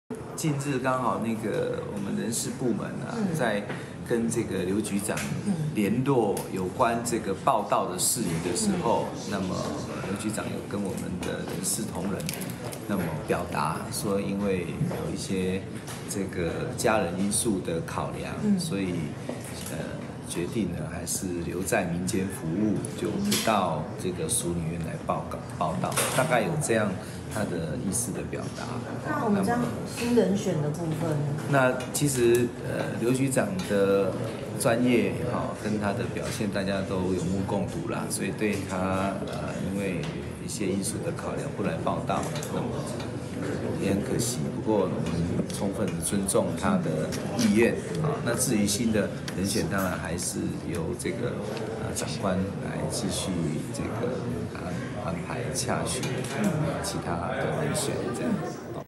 健保署副署長人選生變 劉宜廉「考量家人」婉拒【最新快訊】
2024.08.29【最新快訊】前桃園市衛生局長「劉宜廉」，原定9月就要接任健保署副署長一職，不過傳出因為感情問題，人事安排生變，來聽一段健保署的最新說明。